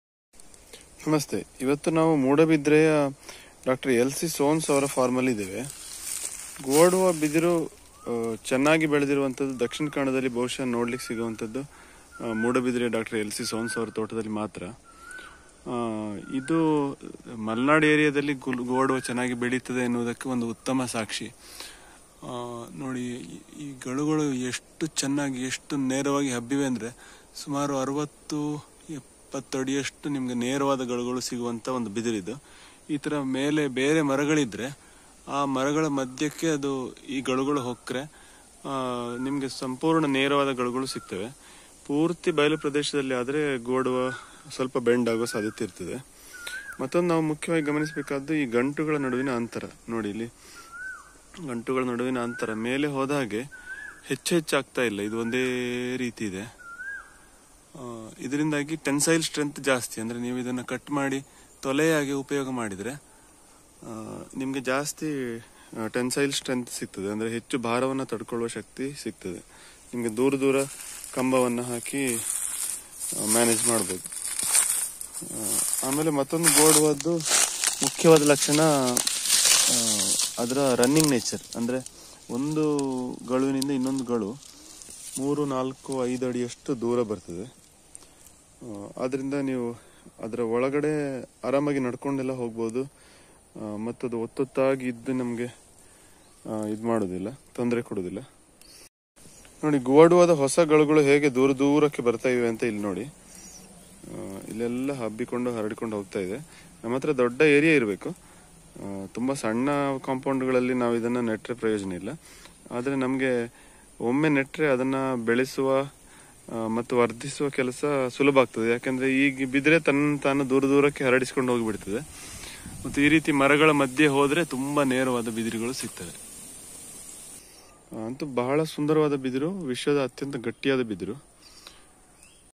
Guadua bamboo grows in India
Guadua Angustifolia is a South American bamboo. It is the world's strongest bamboo. Early plant collectors have proven that it grows well in India, Karnataka as well. Please check the video for details.
At Kaje vrukshAlaya nursery, limited stock of saplings are available (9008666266).